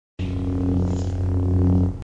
Adobe After Effects lightsaber
join the fanclub,
third day with after effects, lightsaber coming along so-so